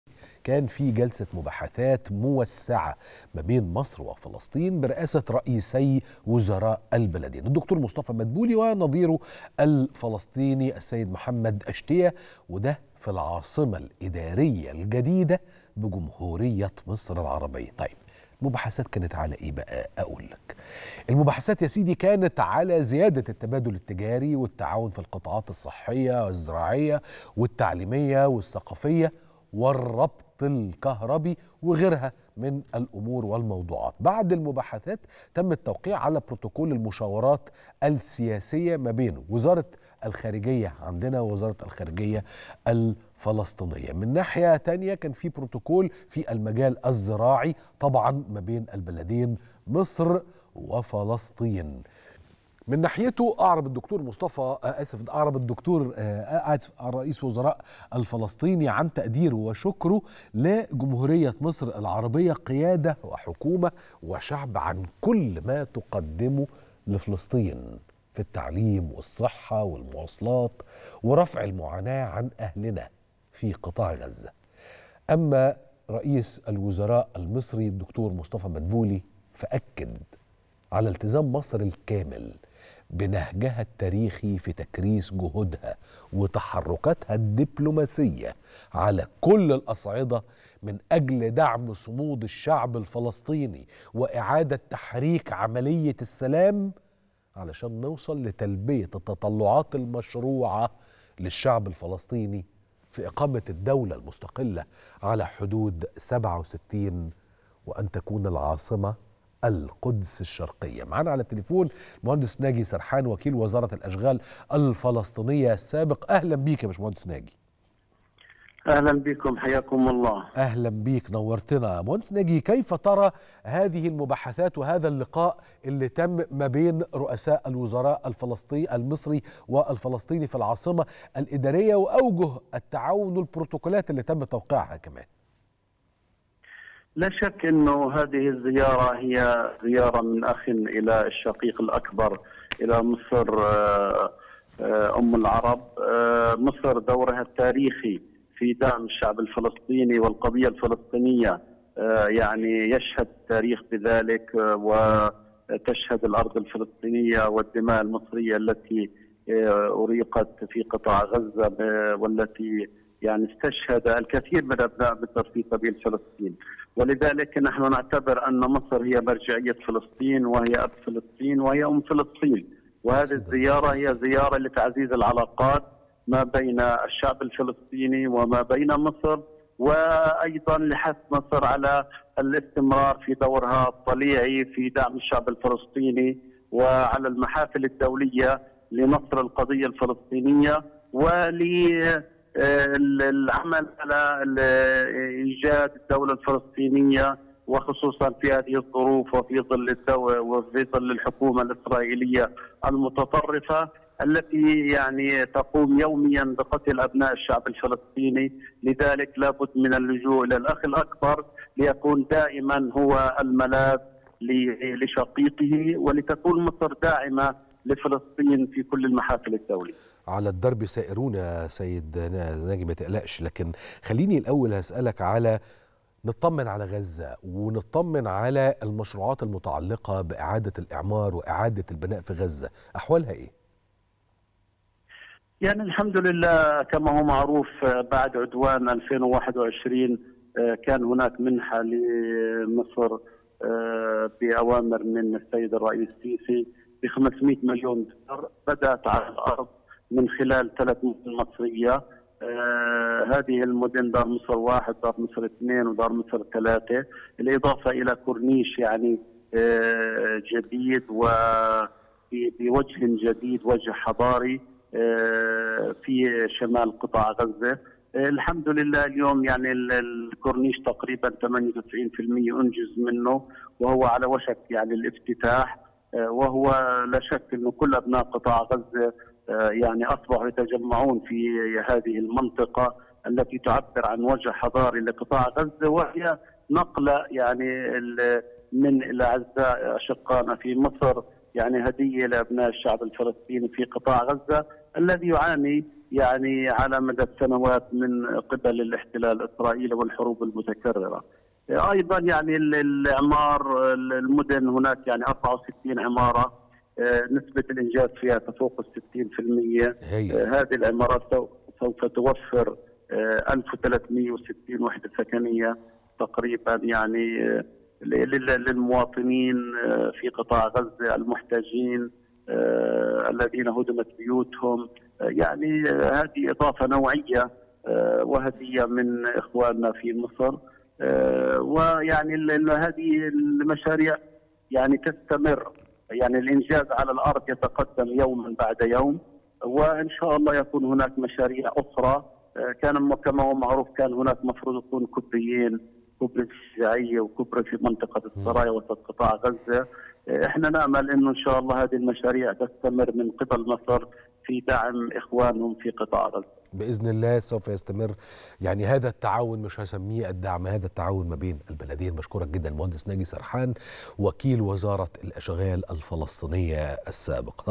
التاسعة | م. ناجي سرحان: المشاريع المصرية في قطاع غزة تتضمن 3 مدن تحمل اسم "دار مصر" ومشروعات طرق
التاسعة | م. ناجي سرحان وكيل وزارة الأشغال الفلسطينية السابق: المشاريع المصرية في قطاع غزة تتضمن 3 مدن تحمل اسم "دار مصر" ومشروعات طرق